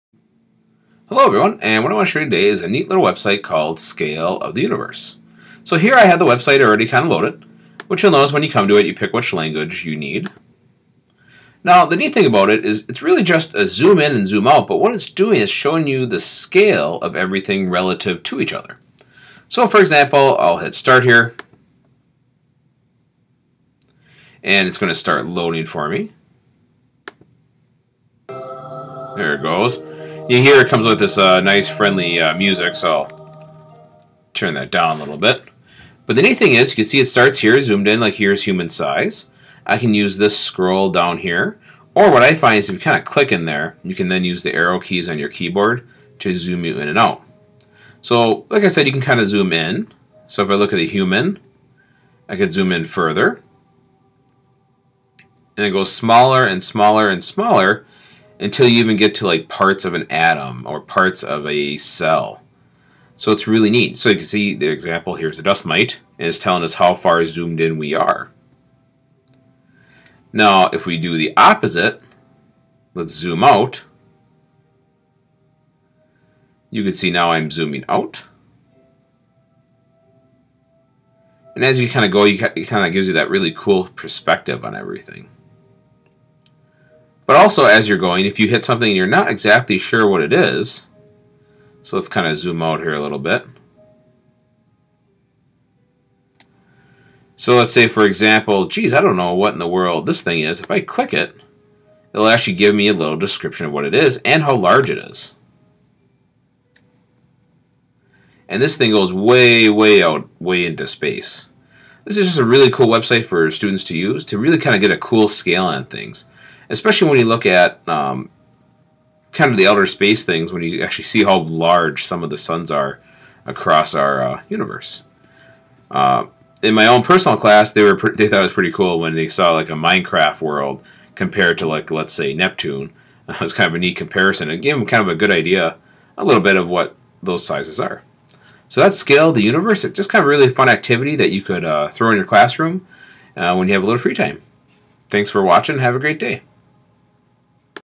Scale of the Universe 2
Scale of the Universe 2 is a great tool to show the size of items from any subject from biology to astronomy. Even if you do not teach any of those classes, it is an amazing website to explore.